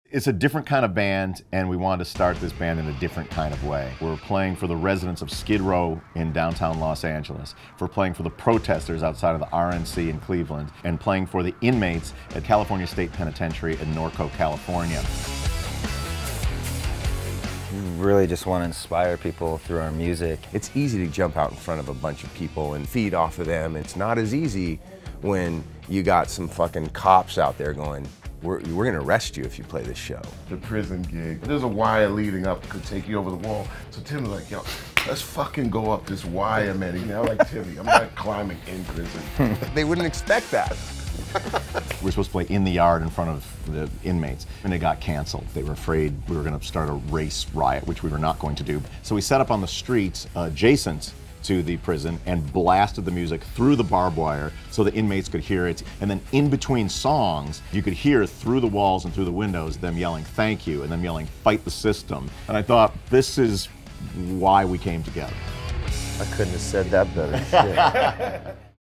Prophets of Rage // Fight The System
Listen to the musical legends from Prophets of Rage talk about the importance of delivering a devastating rock show and what it really means to rage against the machine.